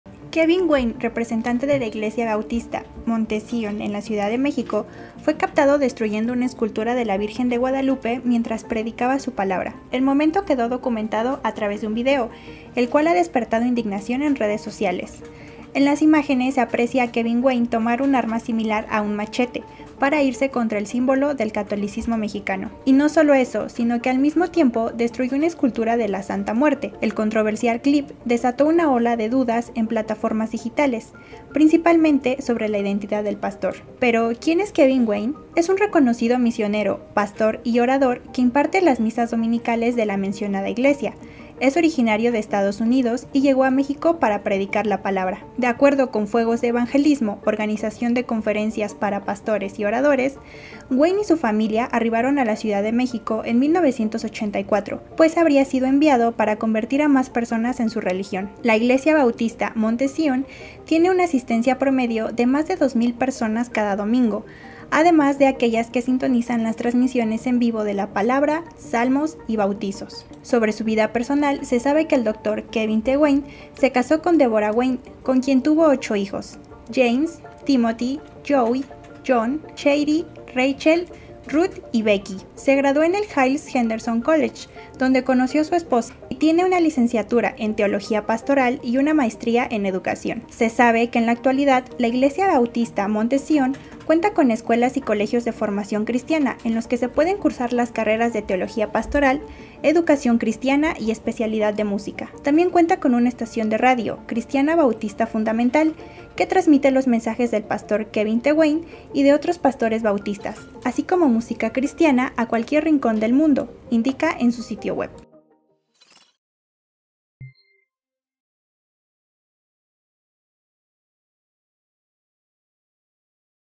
¿Qué sabemos de Kevin T. Wynne?, pastor que destruyó imagen de la Virgen de Guadalupe
El pastor Kevin T. Wynne causó indignación tras romper una escultura de la Virgen de Guadalupe y de la Santa Muerte